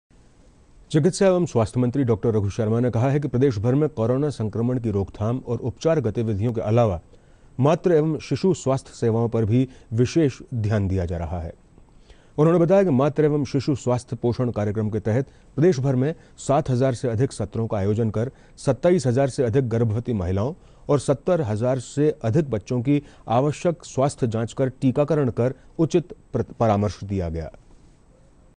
कोरोना के साथ ही मातृ—शिशु स्वास्थ्य सेवाओं पर भी है विशेष फोकस : चिकित्सा मंत्री
चिकित्सा एवं स्वास्थ्य मंत्री डॉ. रघु शर्मा ने कहा है कि प्रदेशभर में कोरोना संक्रमण की रोकथाम और उपचार गतिविधियों के अलावा मातृ एवं शिशु स्वास्थ्य सेवाओं पर भी विशेष ध्यान दिया जा रहा है। उन्होंने बताया कि मातृ एवं शिशु स्वास्थ्य पोषण कार्यक्रम के तहत प्रदेशभर में 7 हजार से अधिक सत्रों का आयोजन कर 27 हजार से अधिक गर्भवती महिलाओं और 70 हजार से अधिक बच्चों की आवश्यक स्वास्थ्य जांच और टीकाकरण कर उचित परामर्श दिया गया।